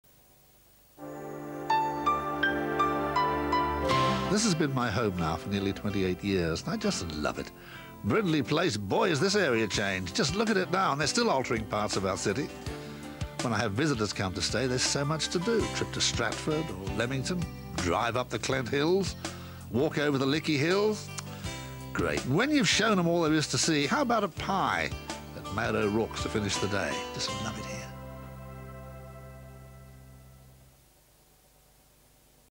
Ed Doolan's Birmingham - BBC WM Advert - HQ
Ed Doolan on his favourite bits of Brum.